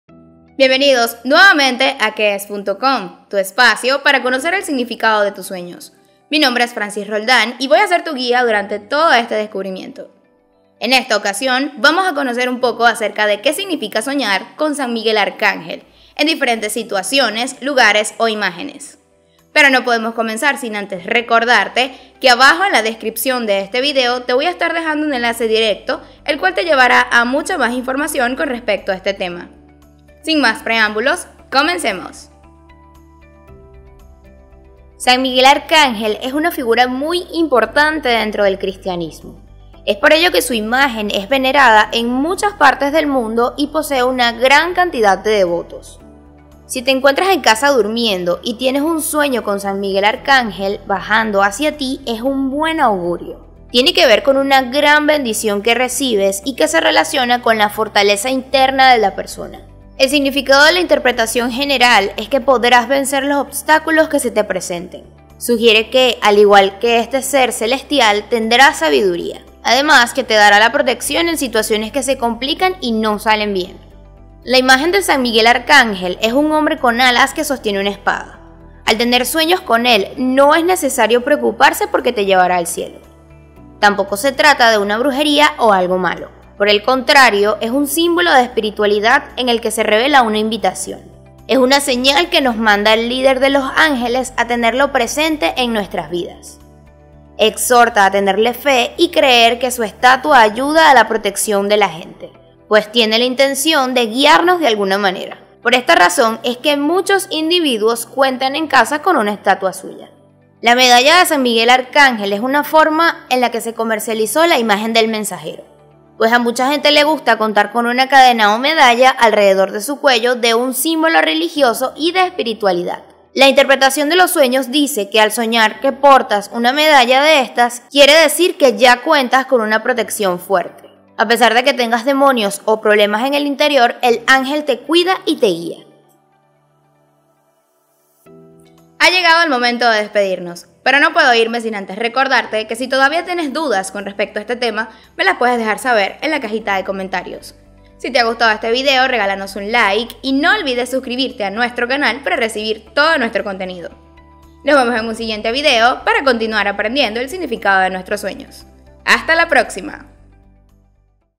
¿Qué significa soñar con San Miguel Arcángel? 🙏 Situaciones, imágenes y lugares 🙏
🌙 ¡Que Significa tu Sueño GRATIS!

¿Qué significa soñar con San Miguel Arcángel? El arcángel San Miguel es una figura muy importante dentro del cristianismo. Es por ello que su imagen es venerada en muchas partes del mundo y posee una gran cantidad de devotos. En este vídeo, exploraremos juntos algunos de los significados más comunes de este sueño. ¡No te lo pierdas!